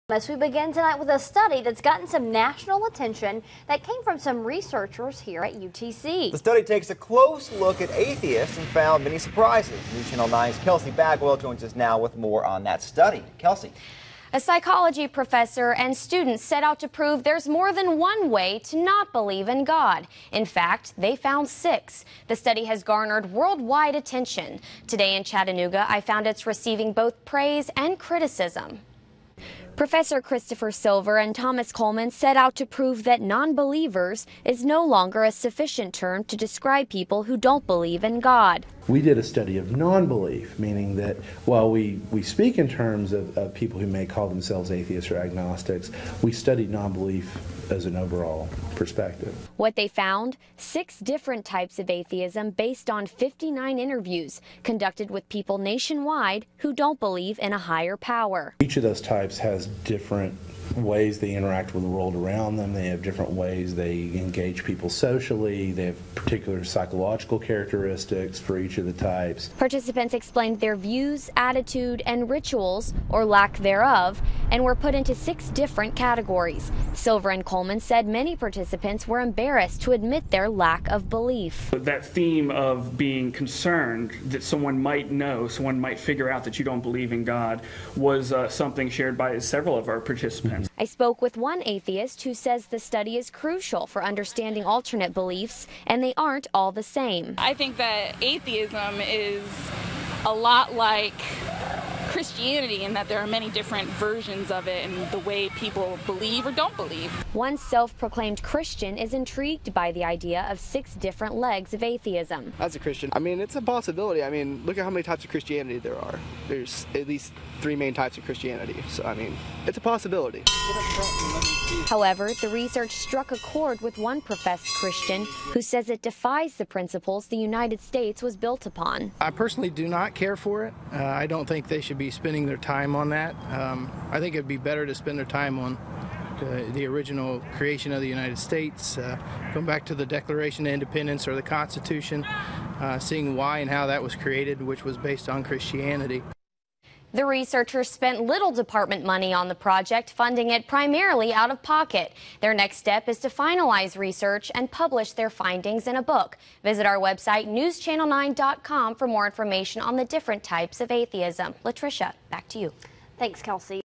UTC Professor Defines 6 Types of Atheism
University of Tennessee at Chattanooga Professor Christopher Silver and psychology student, Thomas Coleman, set out to prove that nonbelievers is no longer a sufficient term to describe people who don't believe in God.

We did a study of disbelief... meaning that while we speak in terms of people who may call themselves Atheists, or Agnostics, we study non-belief as an overall perspective, said Professor Silver.

What they found? Six different types of Atheism based on 59 interviews conducted with people nationwide who don't believe in a higher power.

Participants explained their views, attitude, and rituals, or lack thereof, and were put into six different categories. Silver and Coleman said many participants were embarrassed to admit their lack of belief

That theme of being a concerned someone might know, or someone might figure out that you don't believe in God, was something shared by several participants, said Coleman.

I spoke with one Atheist who says the study is crucial for understanding alternate beliefs, and they aren't all the same.

I think that Atheism is a lot like Christianity in that there are many different versions of it in the way people believe or don't believe, said Courtney Guevara.

One self-proclaimed Christian is intrigued by the idea of six different legs of Atheism.